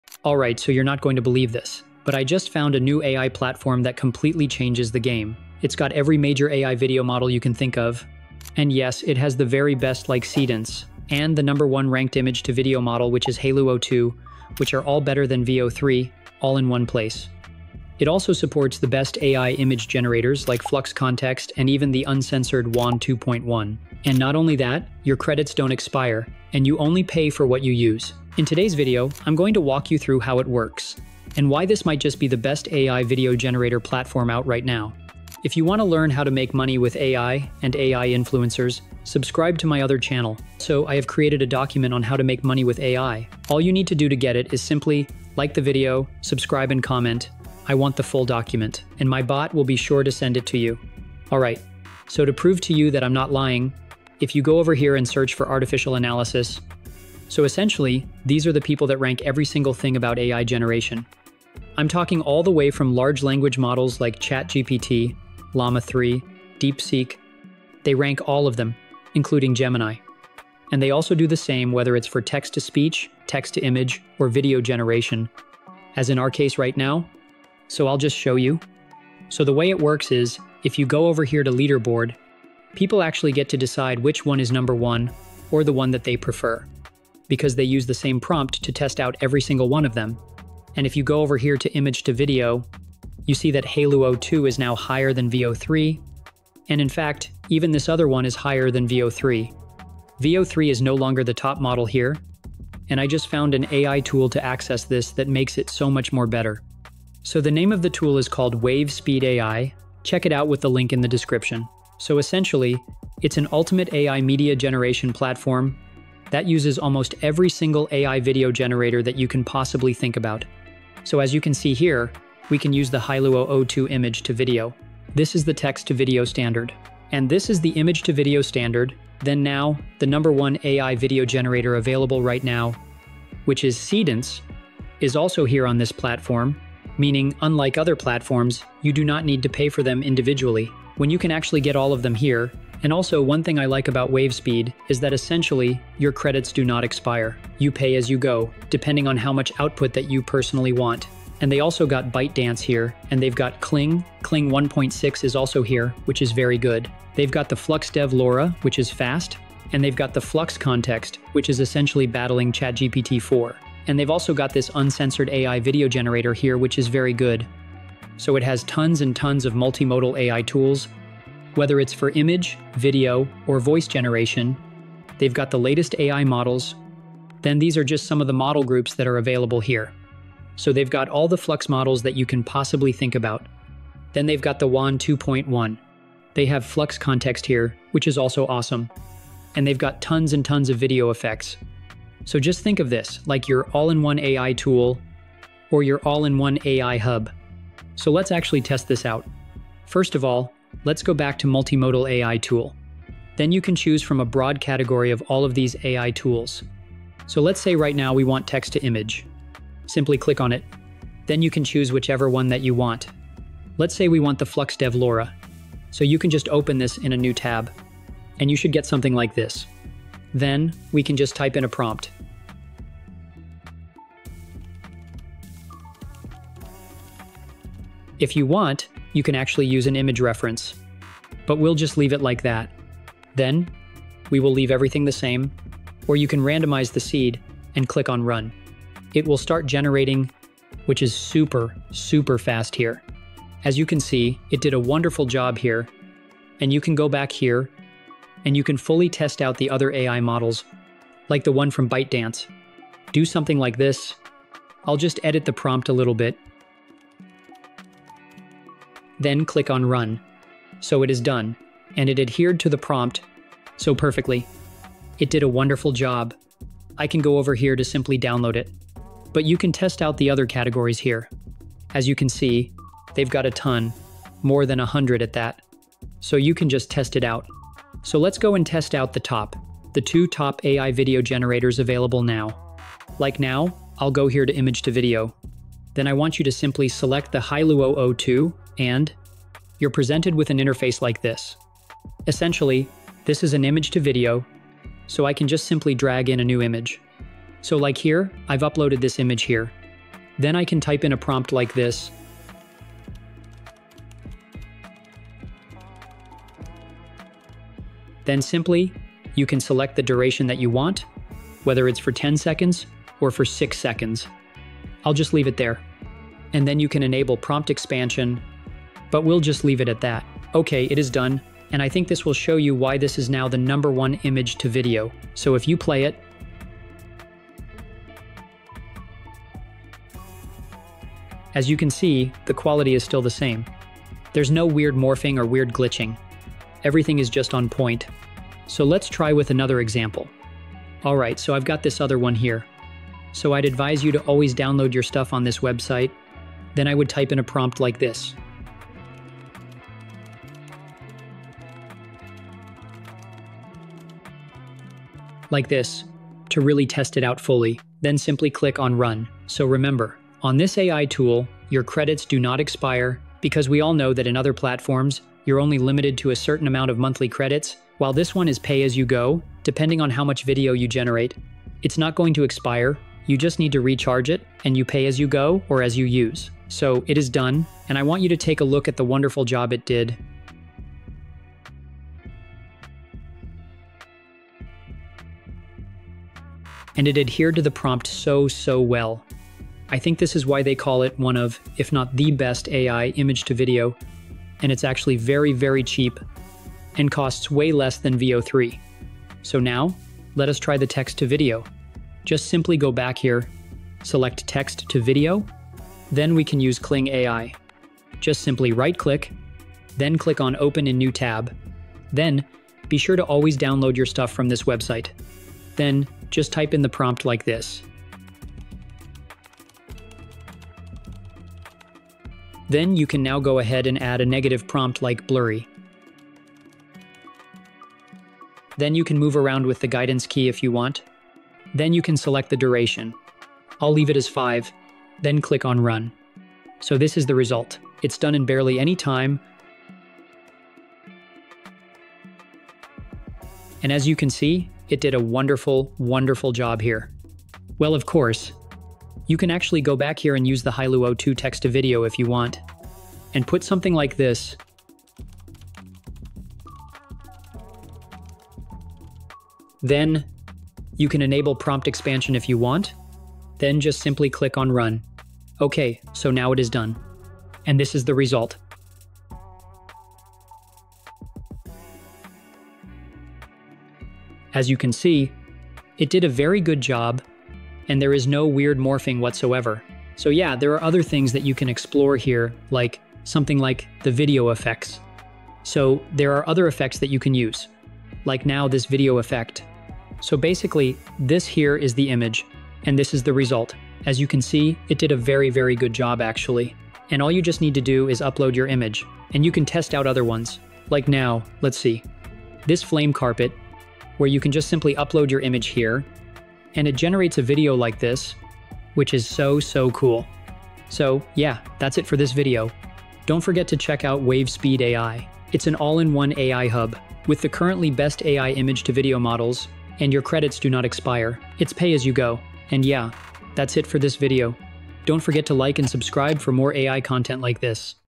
5 TIMES BETTER THAN VEO 3: New AI Video Gen Destroys Veo 3 with FREE Credits || Wavespeed ai
🚀 I just discovered the ultimate all-in-one AI video generation platform, that have models that are beating Veo  3!
In this video, I’ll walk you through how to use it, test out top models like Hailuo 02, Seedance, Kling and show why some models are now ranked higher than View 3 in AI video generation.
From image-to-video, text-to-video, to insane video effects, this platform has it all, and your credits never expire.

🔥 Whether you’re into AI image, video, or voice generation — this is the place to be.